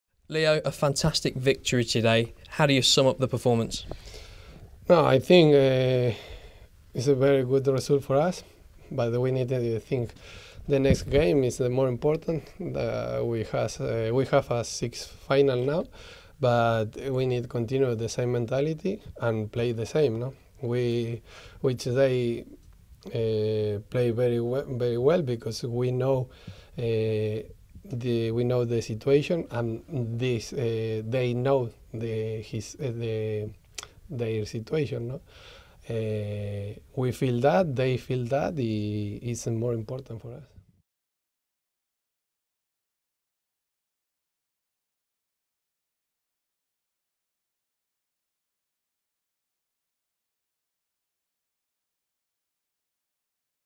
Ulloa On Swansea Win
Leonardo Ulloa gives his thoughts following the 2-0 victory Swansea City on Saturday.